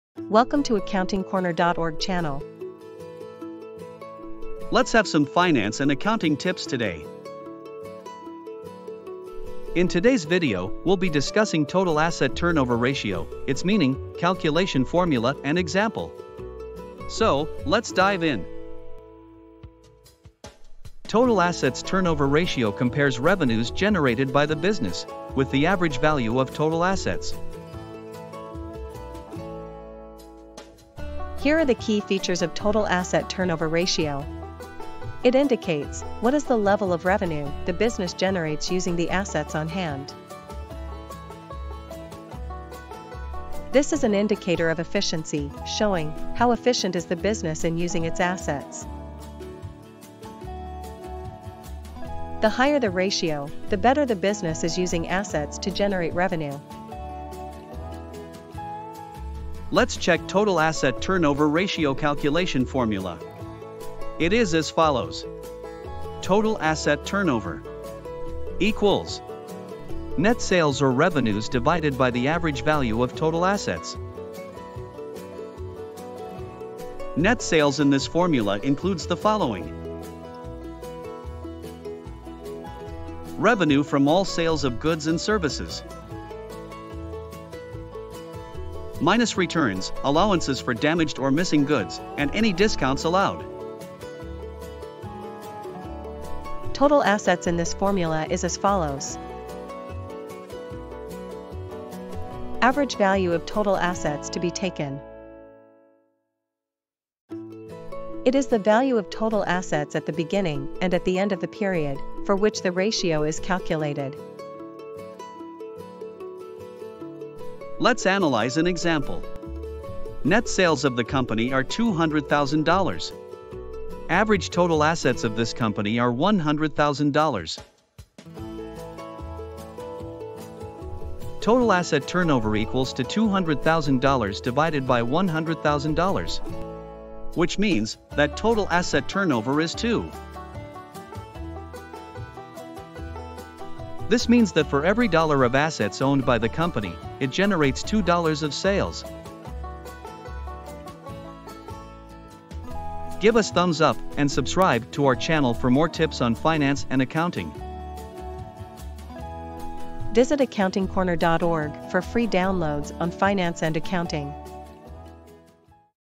Total Asset Turnover Ratio, Formula | AccountingCorner.org
In this video explore: Total Asset Turover Ratio, Formula, Example, Key Features